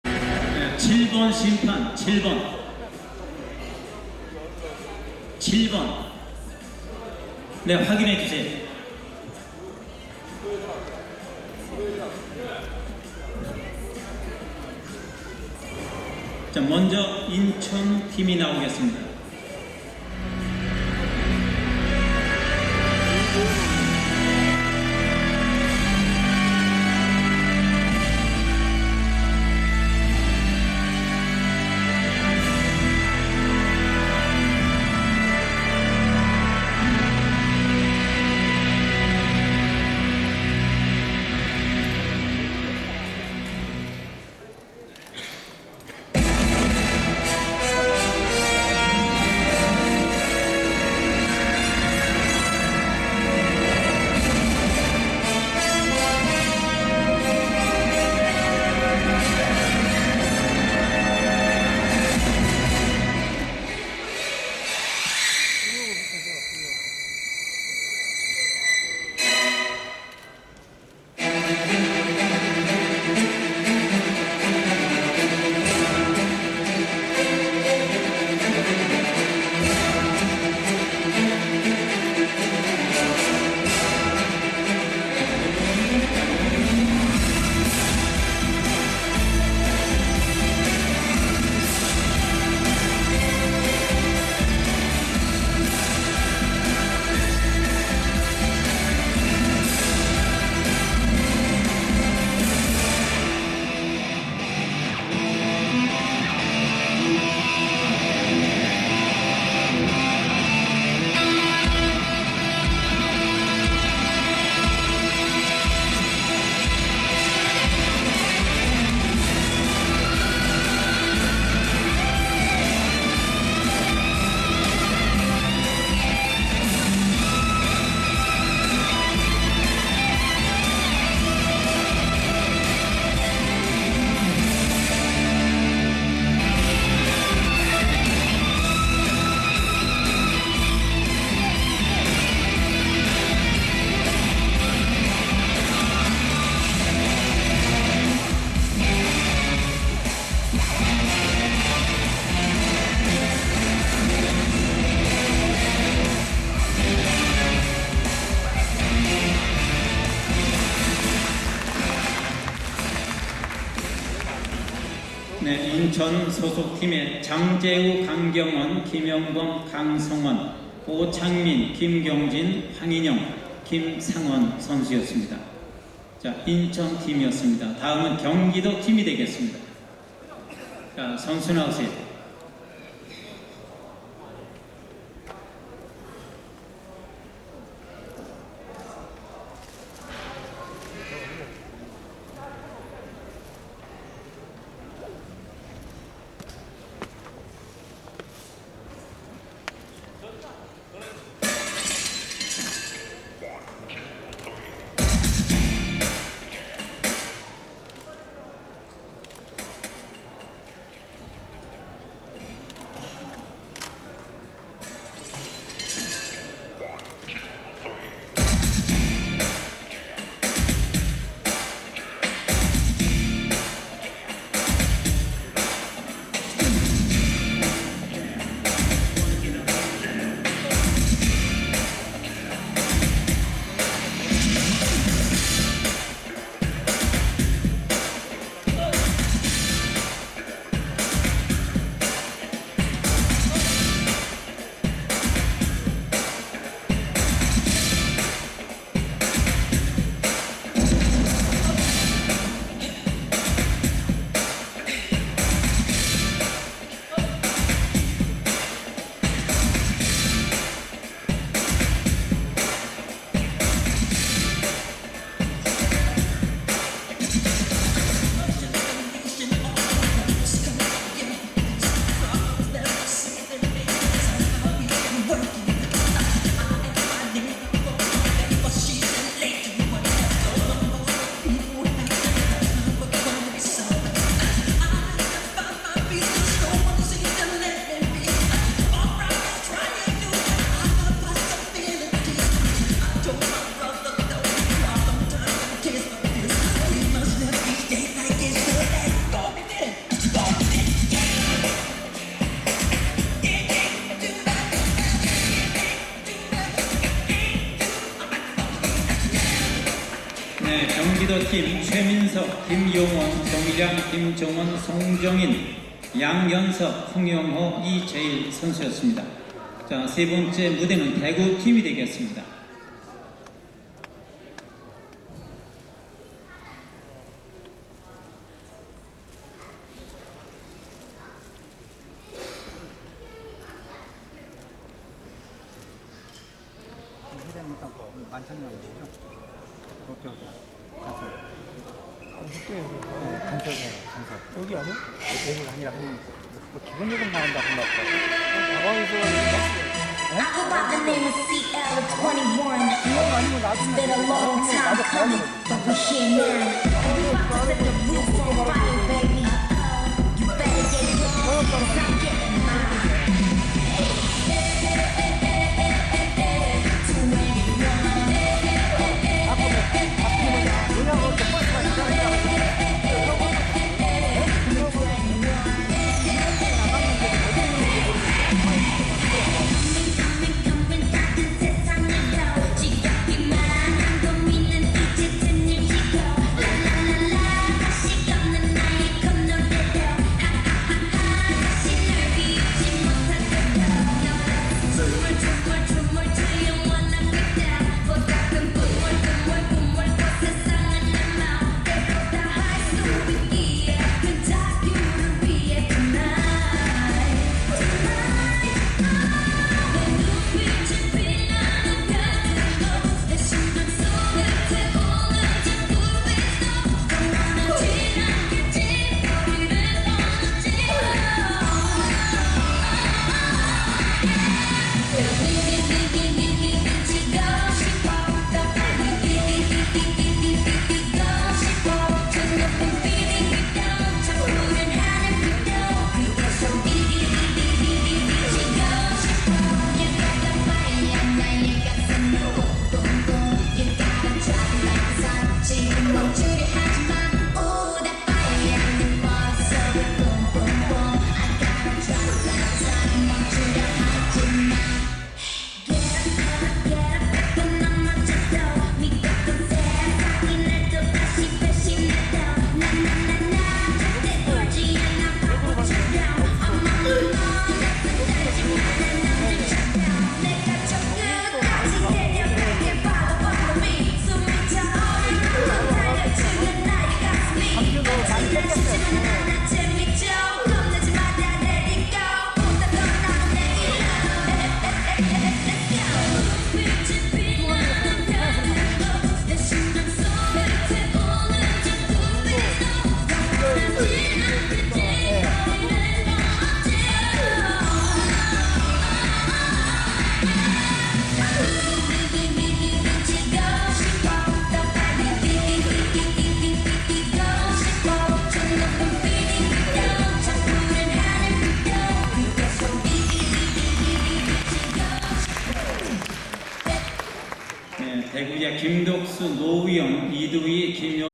Korean Nationals 2010 - Team Posing Highlights (1)
Appearances:
0:45 Kang Kyung Won 강경원 (1st Group)
5:40 Kim Myung Sub (3rd Group)

Editor's Note:
This collection of clips is uploaded by proxy for Yan Wei.
(Recording and copyrights belong to Yan Wei.)